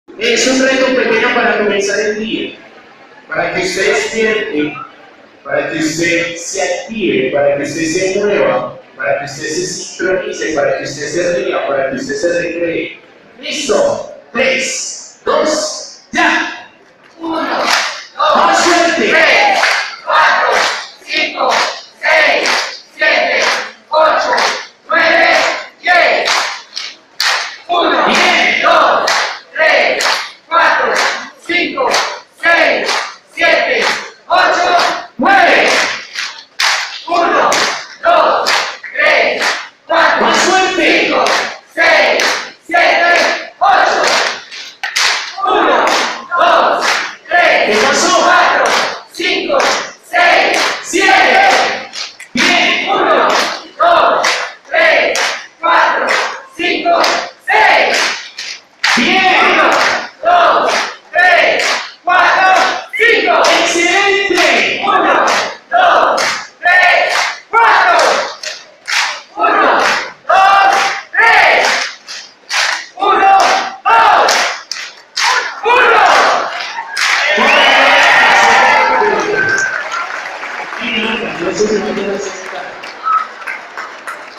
Activación Activo Mi Vida
En diferentes escenarios como eventos empresariales y de ciudad, se disfruta de las activaciones del programa Activo Mi Vida que desarrolla desde el Parque de la vida con la Secretaría de Salud de Medellín.